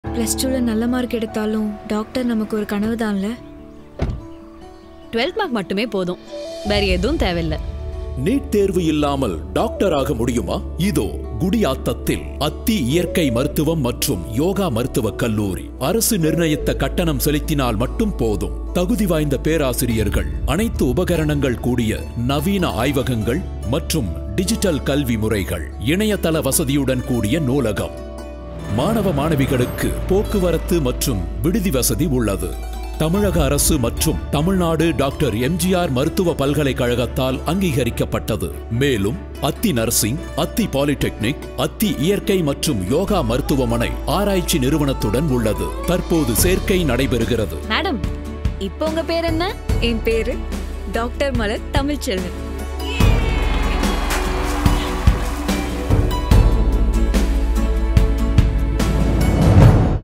Atthi Naturopathy Medical College | BNYS | NEET EXEMPTED | VELLORE
ATTHI NATUROPATHY MEDICAL COLLEGE :
Melpatti Road, Kakathoppu, Gudiyattam, Vellore District- 635 806.

NEET EXEMPTED.

Admission is based on 12th marks.

Physics, Chemistry must. Biology or Botany, zoology.

*Medical course equivalent to MBBS.*

Approved by Tamilnadu Government Atthi Naturopathy Medical College is conducting BNYS (BACHELOR OF NATUROPATHY AND YOGIC SCIENCES), Five and Half years (5 ½ year) Medical Degree course in Naturopathy and Yoga comprising 4 ½ years of course of study and a one-year Compulsory Internship.

Production: STAR 9
Star group of companies
Arthick Jayam

Actress: Gabriella Charlton, Abi Nakshathra.
Director: Sanjay Nirmal
DOP: S.K. Sures Kumar
Edit: Deepak S.Dwaraknath